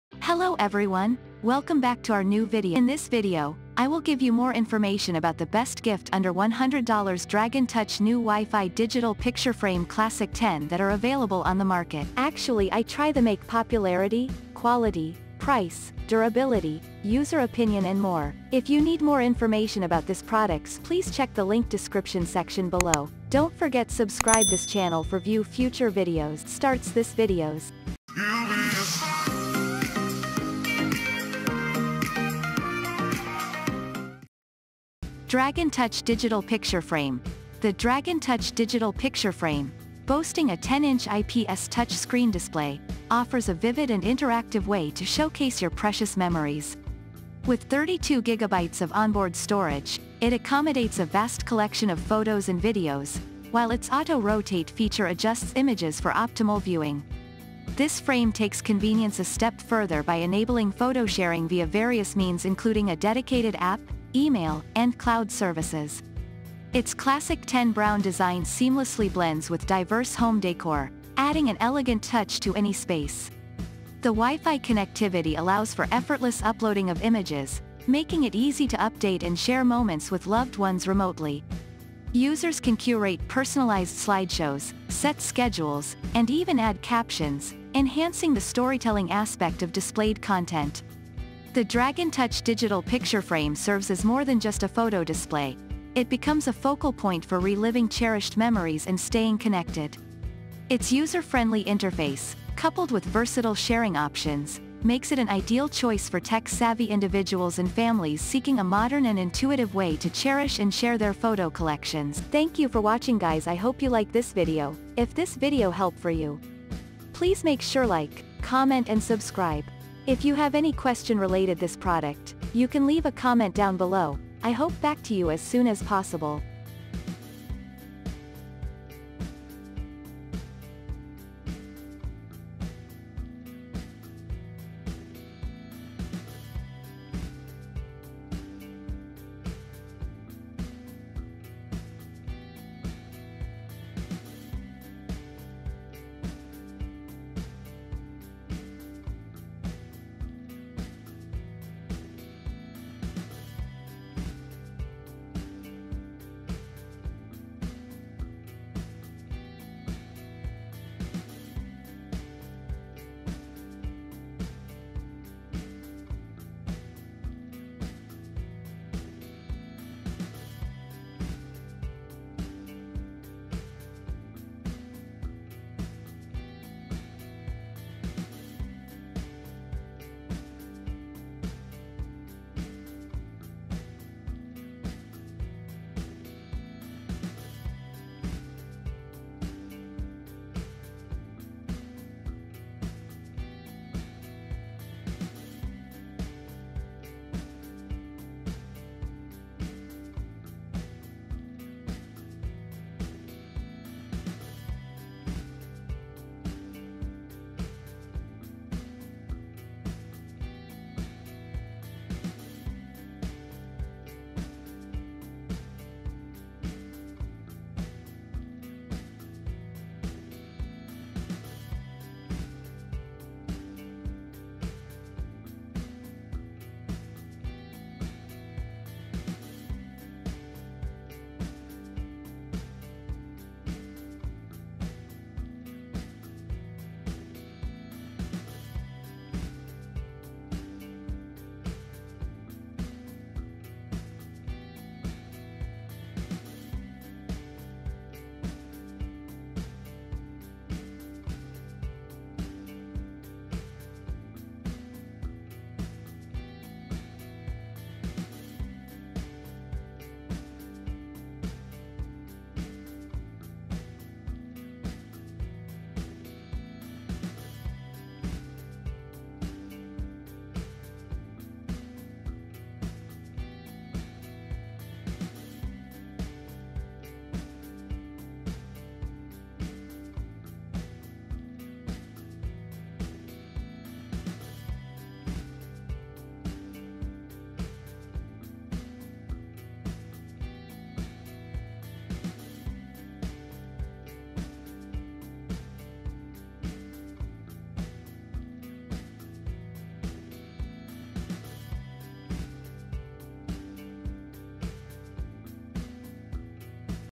Best Gift Under $100 Dragon Touch NEW WiFi Digital Picture Frame Classic 10 Review
☛ All The Links to Best Gift Under $100 listed in this Video:-

Dragon Touch Digital Picture Frame

In This Video, I listed the Best Gift Under $100 That are available in the market. I made this list based on my personal research, and I tried to list them based on their price, quality,  and many more things.